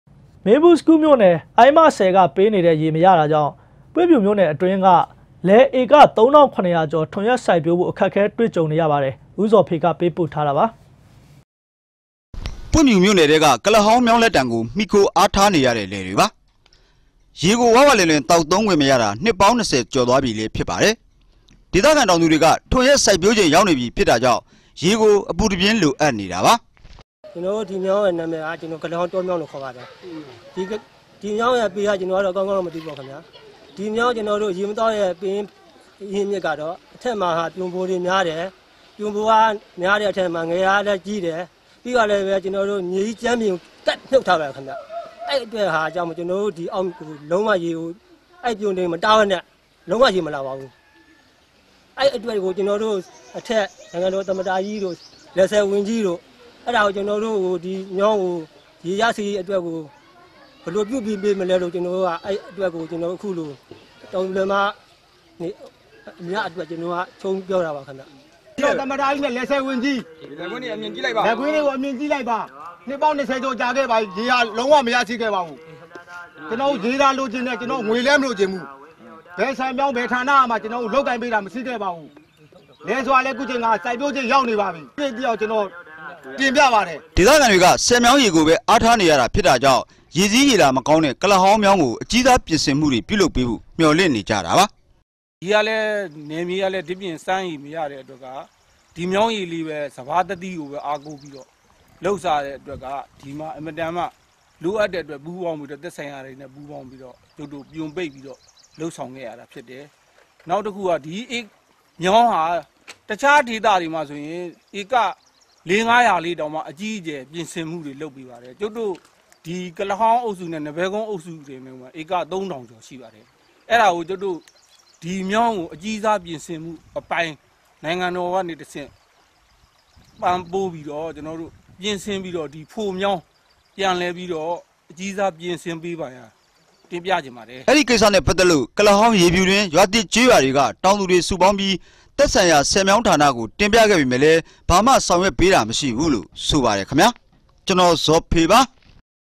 DVB -မင္းဘူး(စကု)ၿမိဳ႕နယ္ အုိင္းမဆည္ကေပးေနတဲ့ ေရမရတာေၾကာင့္ ထြန္ယက္စုိက္ပ်ဳိးဖုိ႔ အခက္ခဲ
DVB TV - မင္းဘူး(စကု)ၿမိဳ႕နယ္ အုိင္းမဆည္ကေပးေနတဲ့ ေရမရတာေၾကာင့္ ပြင့္ျဖဴၿမိဳ႕နယ္အတြင္းက လယ္ဧက ၃၇၀၀ ေက်ာ္ ထြန္ယက္စုိက္ပ်ဳိးဖုိ႔ အခက္အခဲေတြနဲ႔ႀကဳံေနရပါတယ္၊ ဦးေဇာ္ေဖ သတင္းေပးပုိ႔ထားပါတယ္။